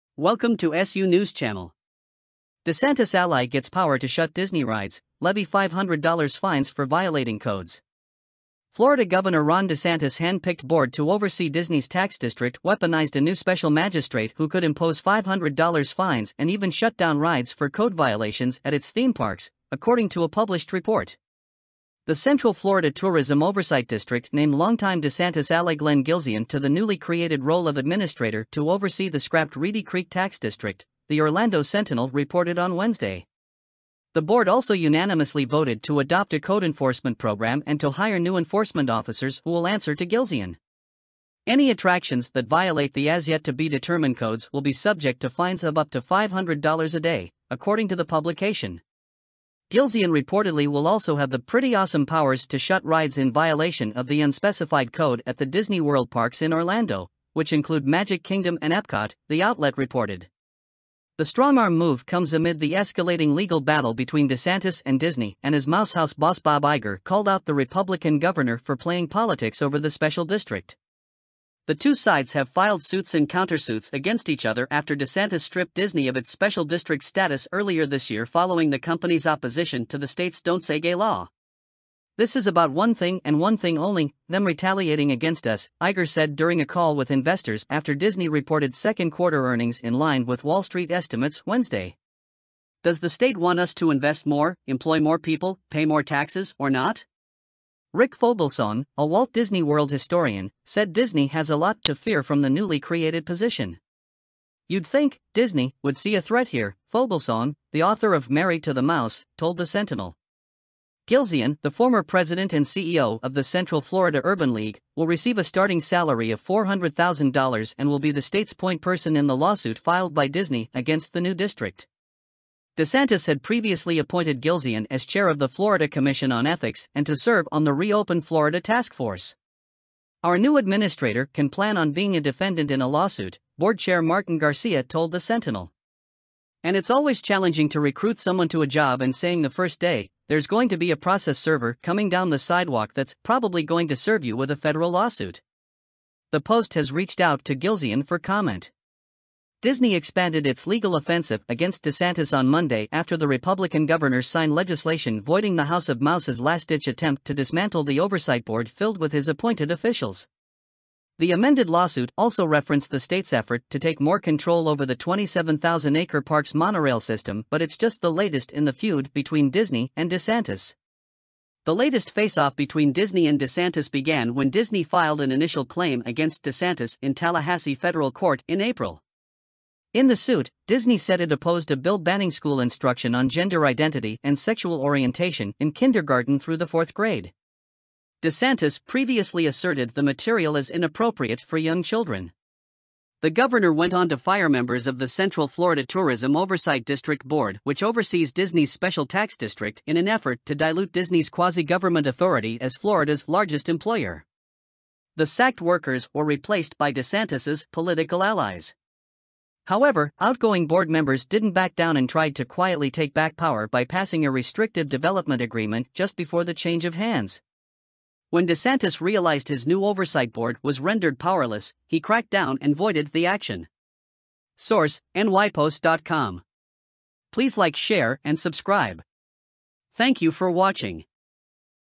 News: DeSantis ally gets the power to shut Disney rides, levy $500 fines for violating codes, SUNews
1. Video Quality
2. Type of Breaking News and News Clip Content that you want.